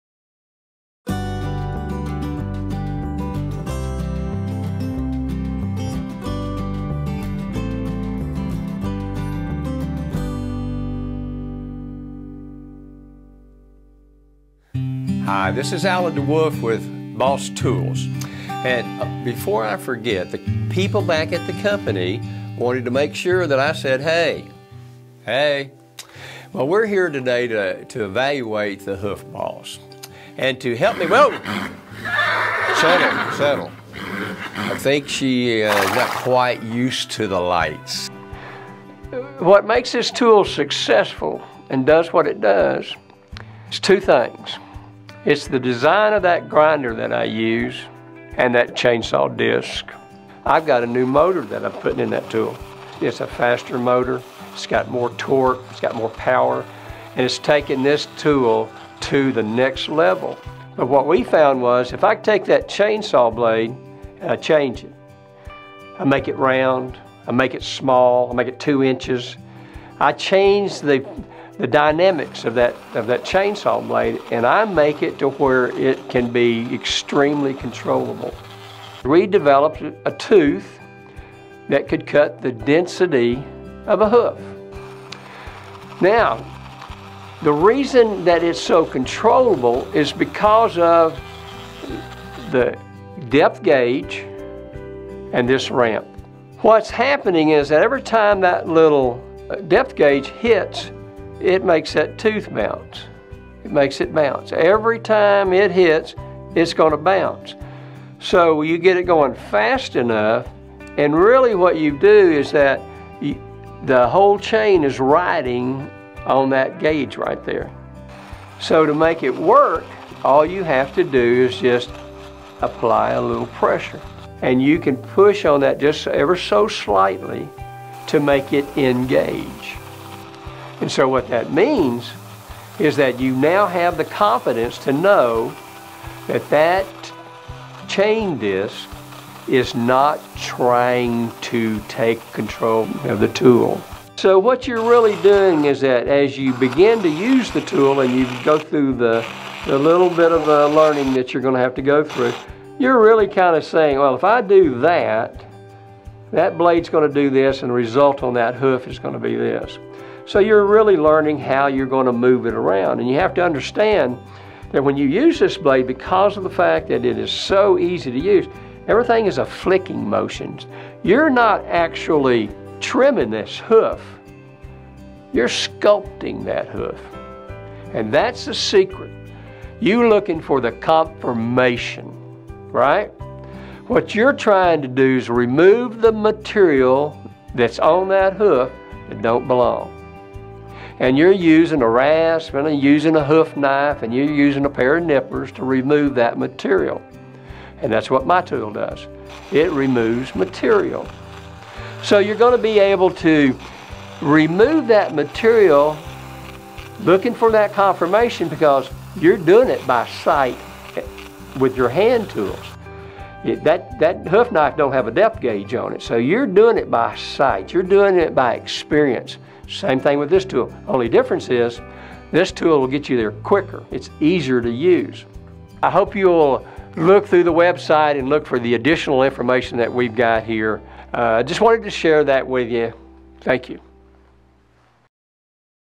HOW IT WORKS - Easy to use Hoof Trimming Tool.
The CEO of Boss Tools, Allen DeWolf, explains how the Hoof Boss works.